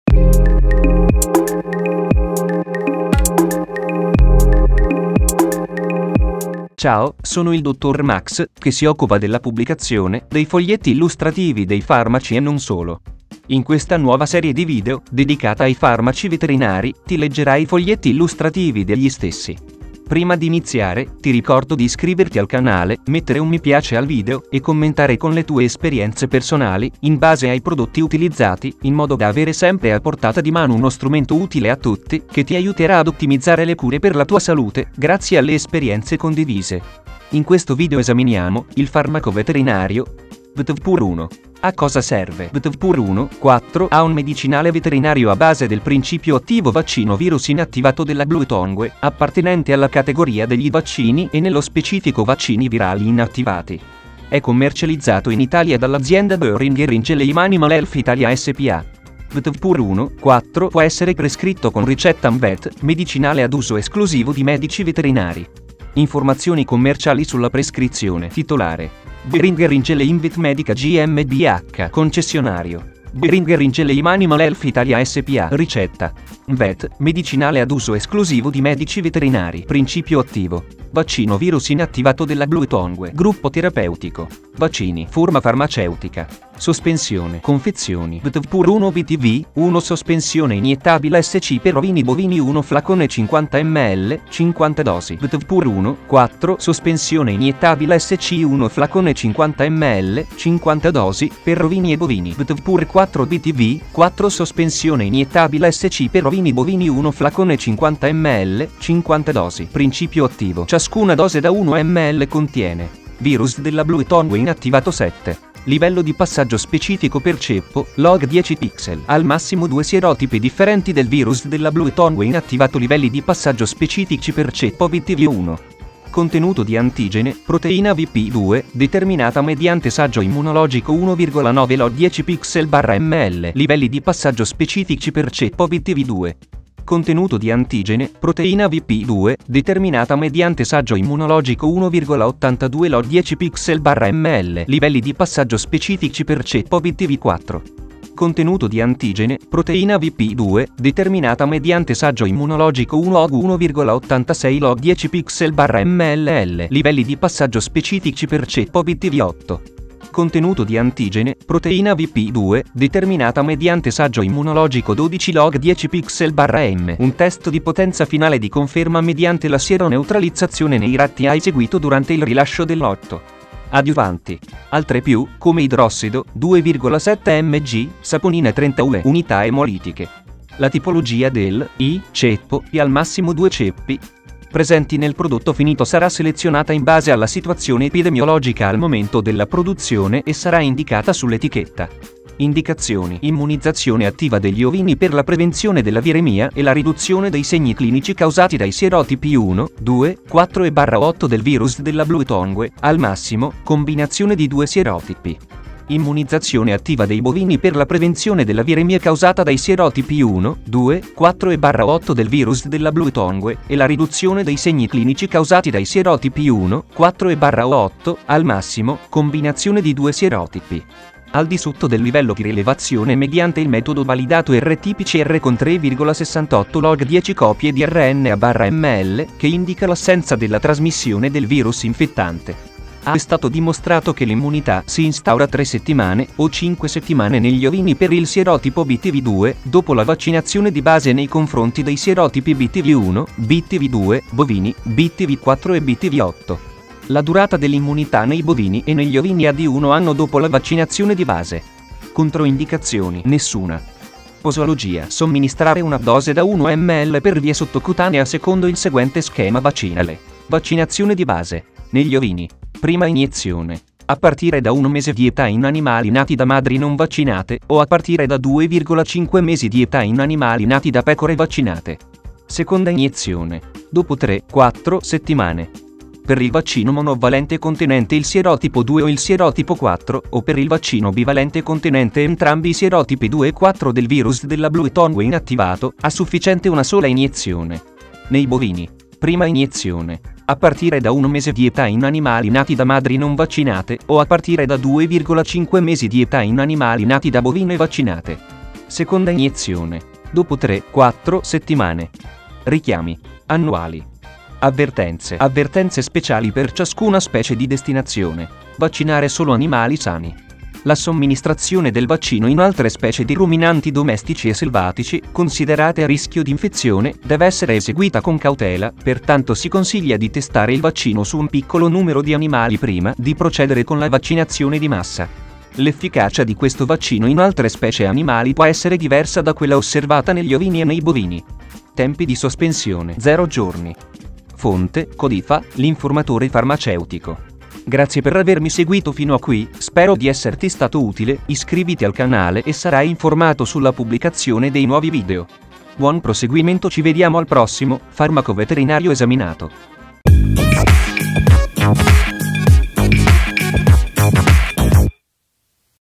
🐶 BTVPUR 1 Farmaco Veterinario a cosa serve ? - Foglietto Illustrativo Salute
Abbiamo letto il Foglietto Illustrativo e parlato del Farmaco Veterinario: BTVPUR 1

Capitoli ⏱
0:00 Introduzione
0:06 Presentazione
0:40 Farmaco Veterinario BTVPUR 1
1:01 A cosa serve ?

BTVPUR 1 Farmaco Veterinario